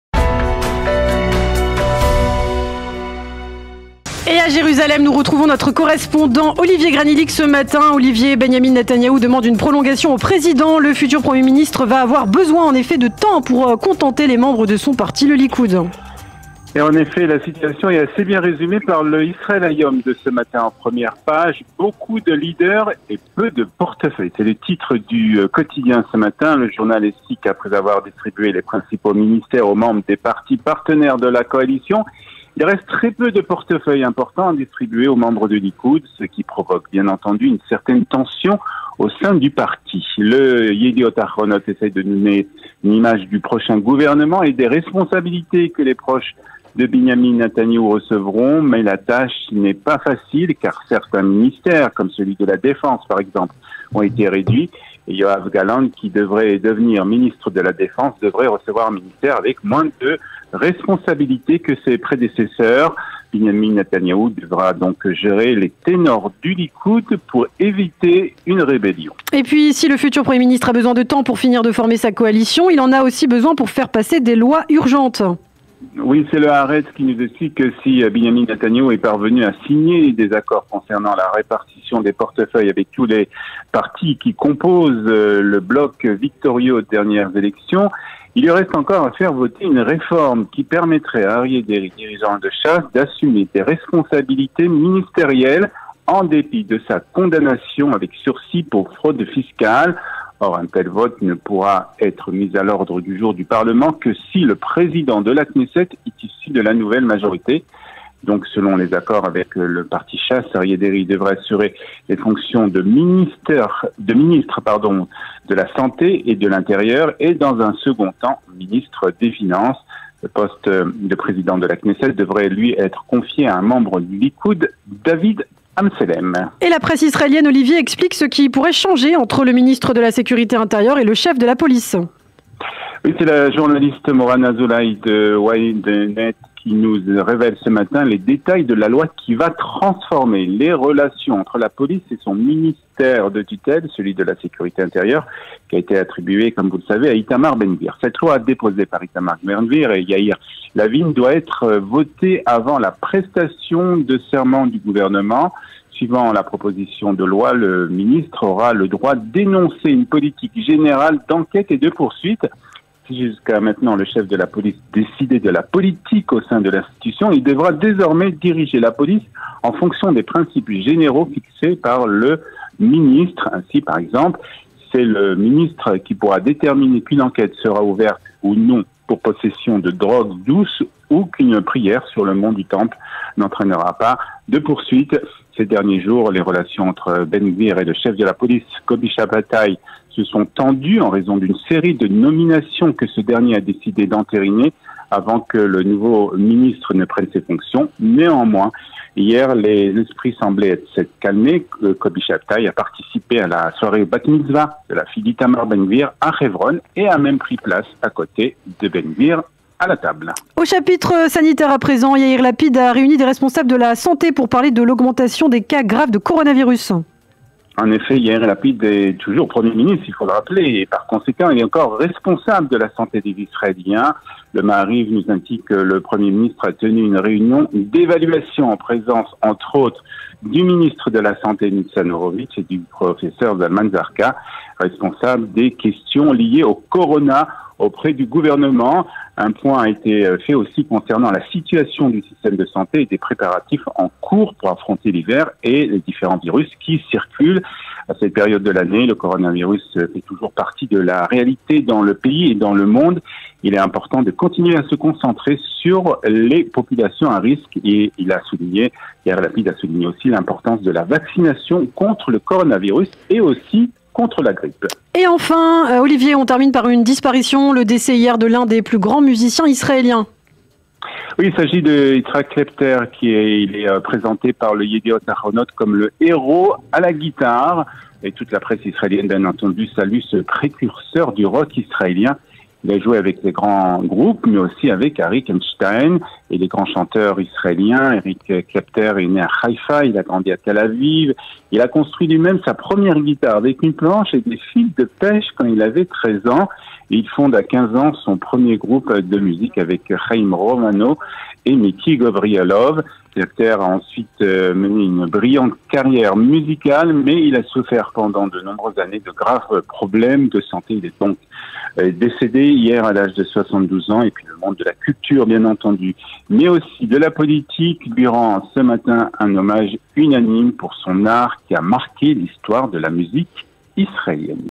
La revue de la presse du 9 décembre 2022
Benyamin Netanyahou demande une prolongation au président. Il va avoir besoin de temps pour satisfaire les membres de son parti.
Yair Lapid a réuni les responsables de la santé pour parler de l'augmentation des cas de coronavirus en Israël.
Le grand musicien israélien, Itzhak Klapter est décédé hier à l'âge de 72 ans.
Écoutez la revue de presse d’Olivier Granilic ce 9 décembre 2022.
Téléchargez des maintenant l’application studio Qualita

Rejoignez la communauté d’entraide francophone en Israël